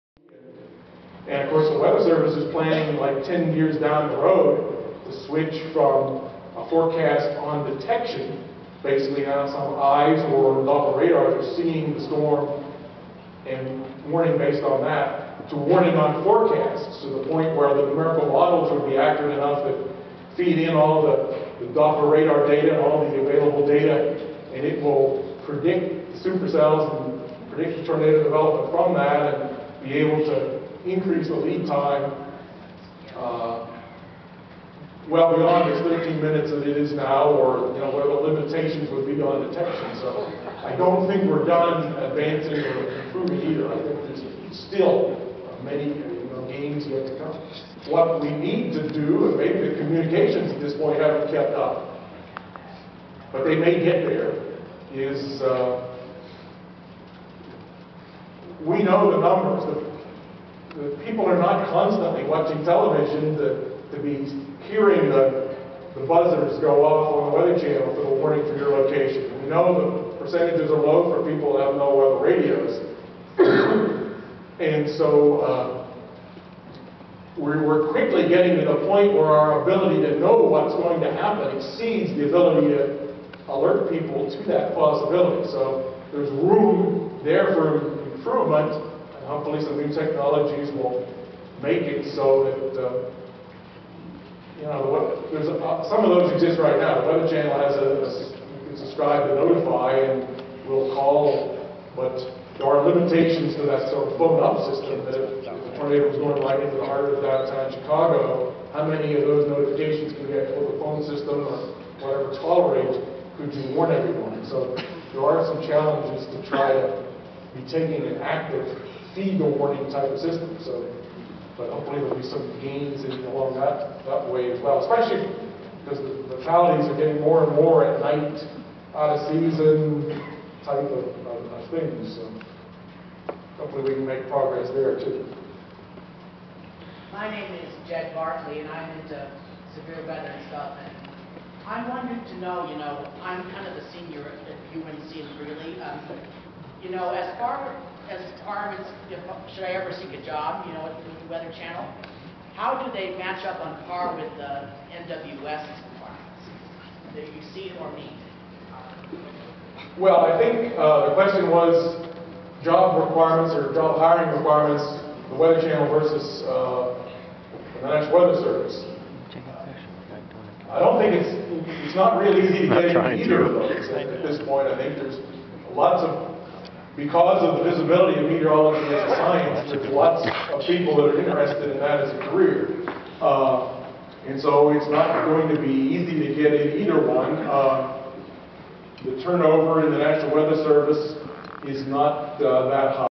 Chaser Con 2010 Keynote Speaker Dr Greg Forbes
This is toward the end of the presentation and he was answering questions at this point.